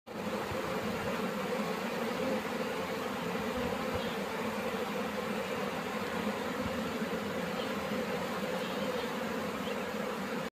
Moving bees into a new hive
moving bees into a new hive by hand scooping.

If you need to move bees into a new hive, it is best to use a bee vacuum or a bee brush to gently encourage the bees to move into their new home. These tools are designed to minimize stress on the bees and to protect the person handling them.
Before moving the bees, make sure that the new hive is set up and ready to receive them. This includes having frames and foundation in place, and ensuring that the hive is level and secure. When moving the bees, be gentle and avoid jostling the hive or disturbing the bees too much.
It is also important to note that moving bees into a new hive should be done during a time when the bees are less active, such as in the early morning or late evening, when they are less likely to be foraging.